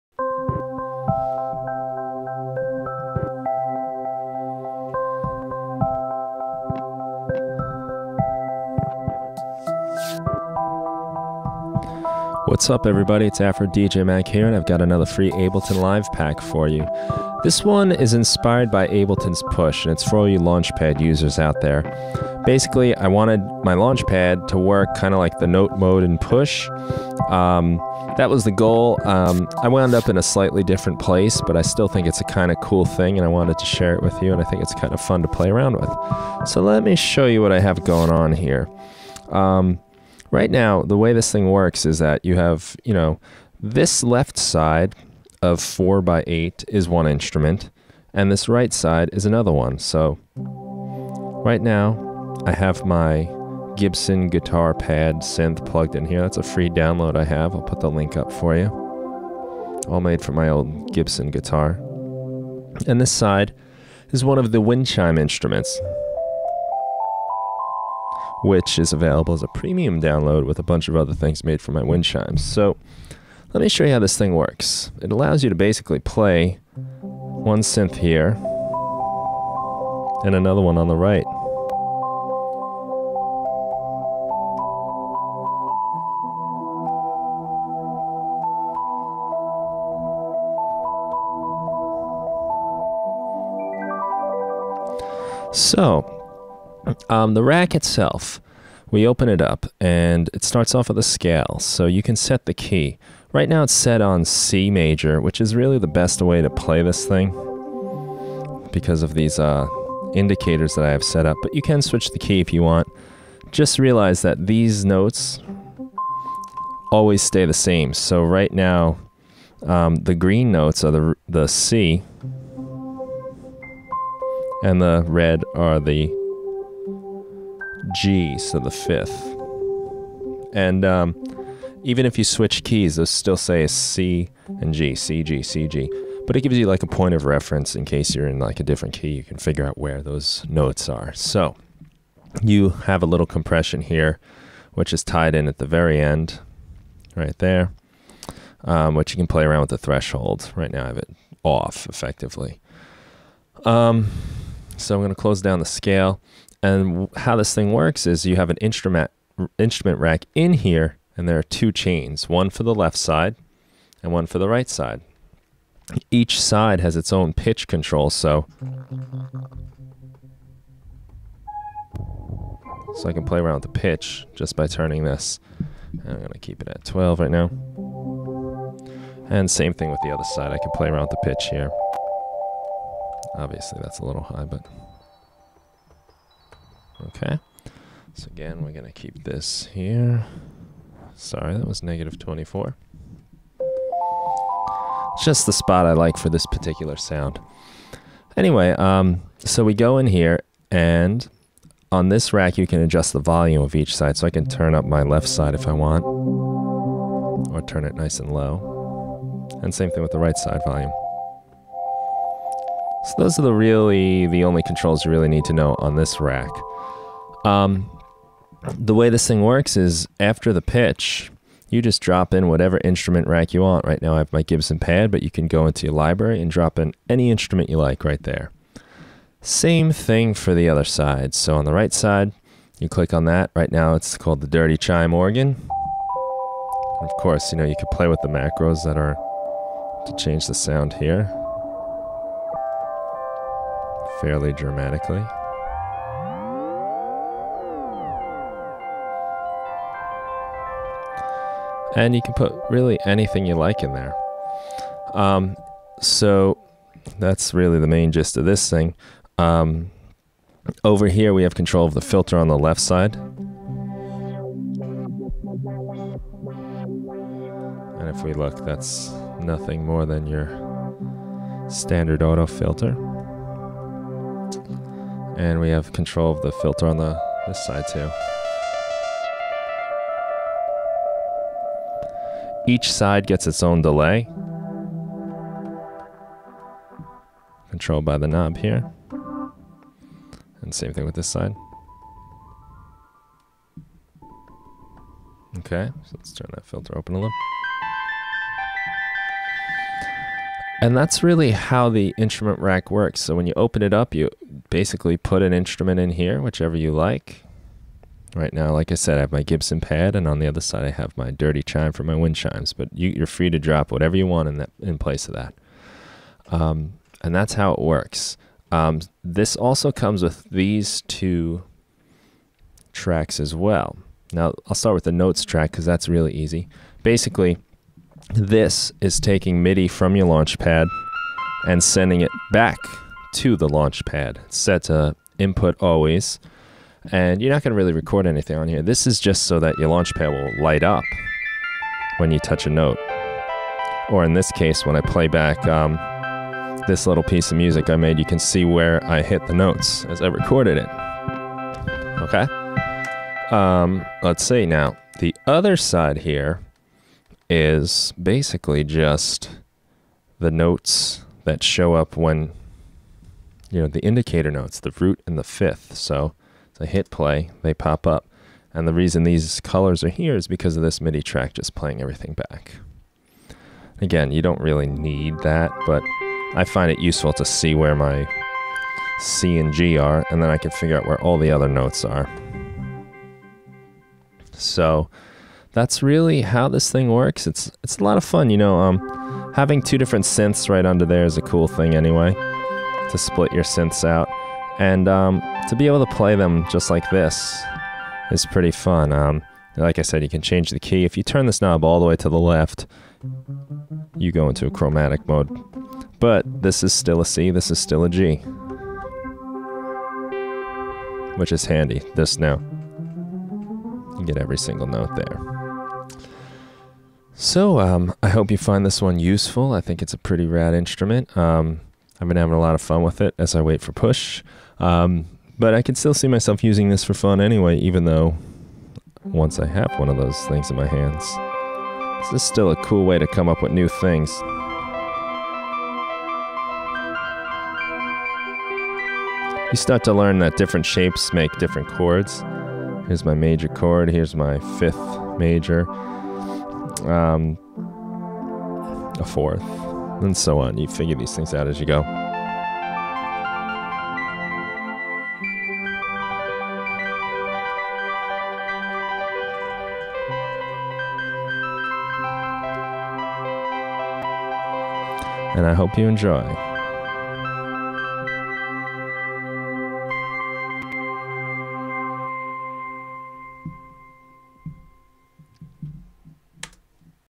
LaunchPad Push Emulation Ableton Instrument Rack
LaunchPad Push Emulation for Ableton Live.

I decided to try and make my Novation Launchpad function like the new Ableton Push.  I've created an instrument rack that allows you to play your LaunchPad in a manner similar to Ableton's Push. It's not an exact replica, in fact it is quite different.  It's a fun way to play around with your LaunchPad and allows you to use put different synths on each side of the instrument.